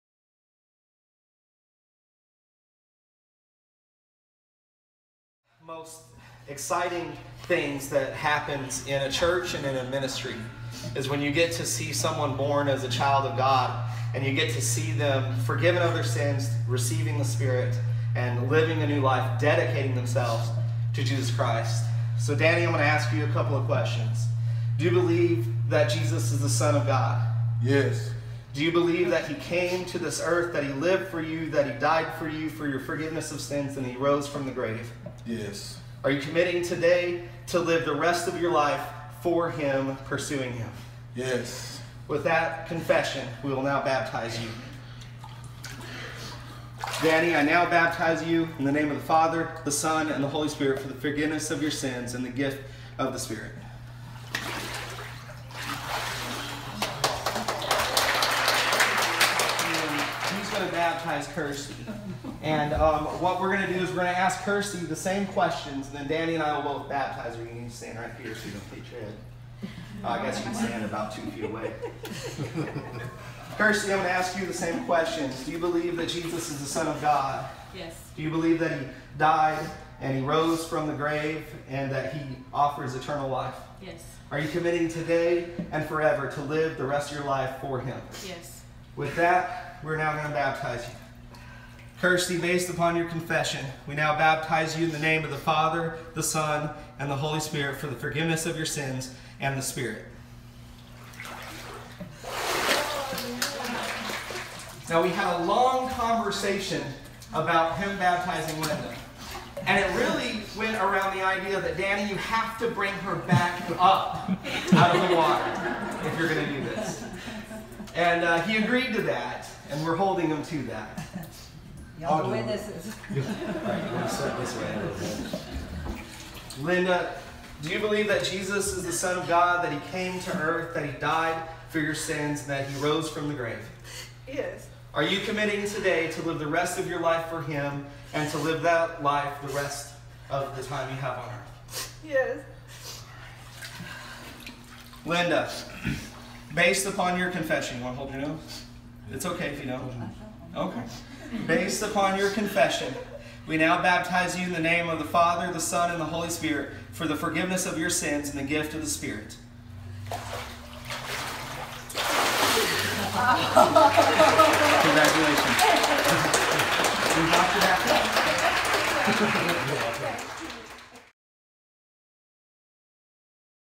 052417 Danny Kerstie and Linda Baptism
Celebrate with us as Danny, Kerstie, and Linda Munoz put Christ on in baptism!